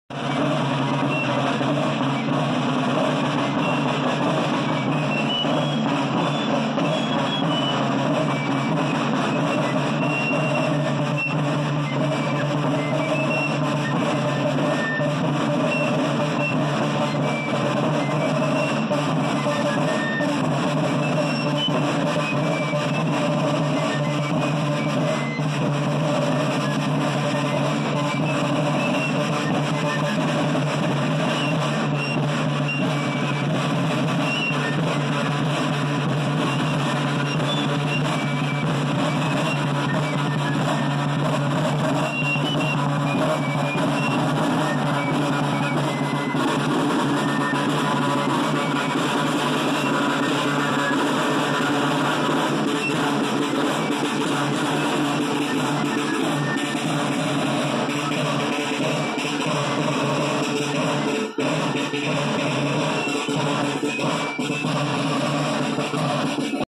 Fusiliers Marins 1810 pas de charge 2021
Tour de la Madeleine 2021.
Fusiliers Marins 1810 et Marins de la Garde.
Pas de charge montée des Grands Sarts à Viesville.